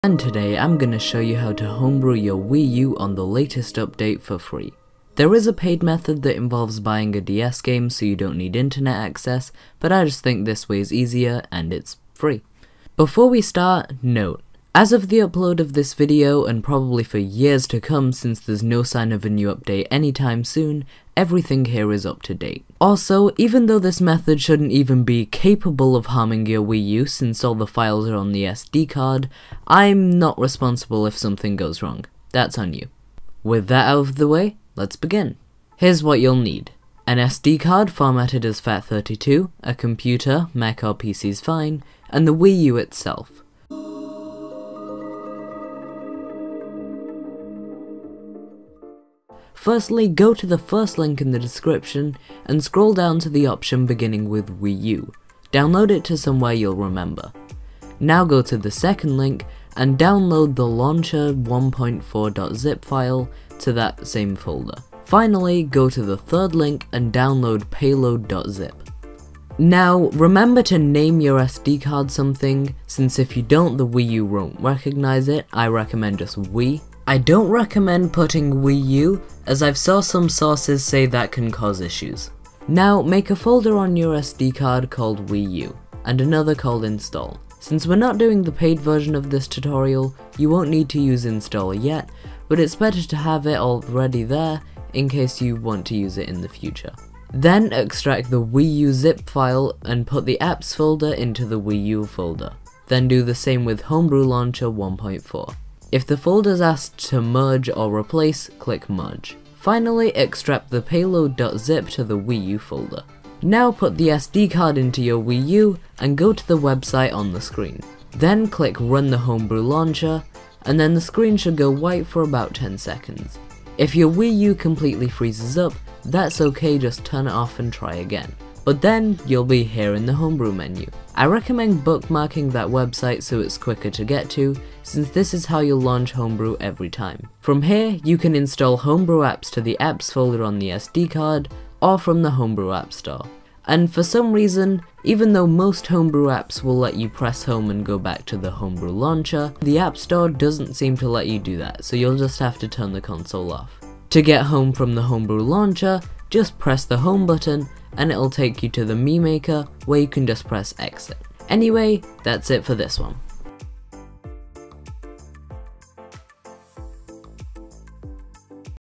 How To Homebrew The Wii U For FREE! - Latest Update
Just follow the immediately below site. At any point, my video could become obsolete

OLD DESCRIPTION
THIS WORKS ON 5.5.4 AND 5.5.3 (currently, 5.5.4 is only on EU systems, however once out on other regions, the process won’t change)

How to homebrew the Nintendo Wii U for FREE on the latest update!

Like he’s proper good at music go check him out

Font: Uni Sans